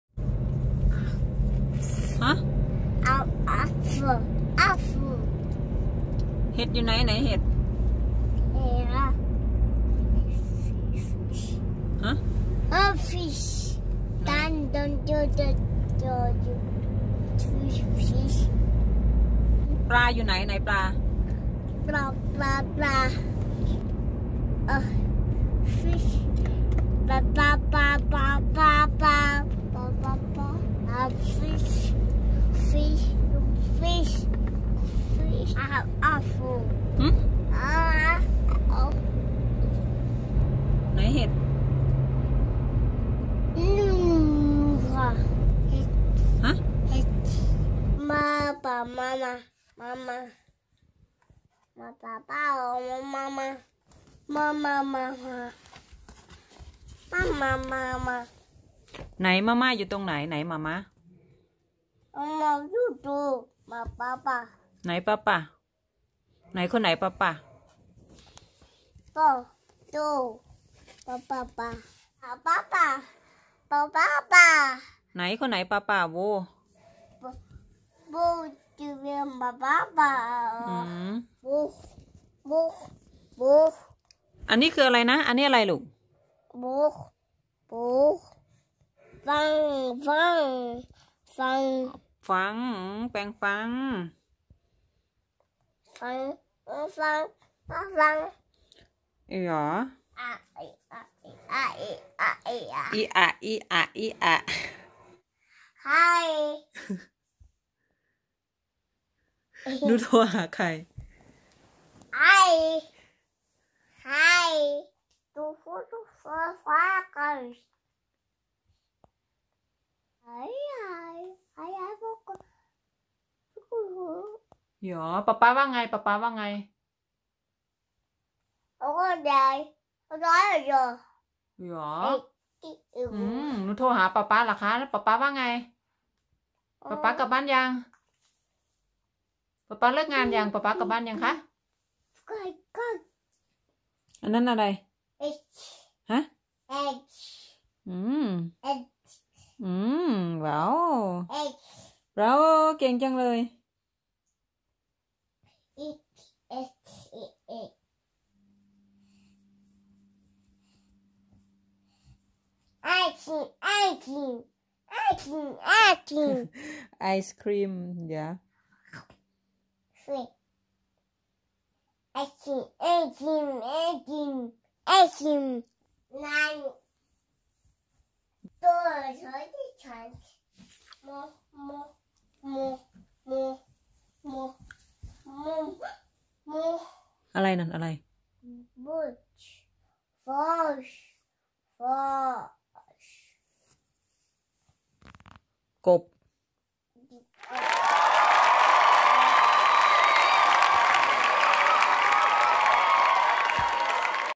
เมื่อหนูหัดพูด(3 ภาษา)น้องธันวาในวัย 1 ขวบ 10 เดือน
เมื่อหนูหัดพูด(3 ภาษา)น้องธันวาในวัย 1ขวบ 10 เดือน หนูจะไหวไหม..?!! ฮ่าๆๆ มาลุ้นกันค่ะ...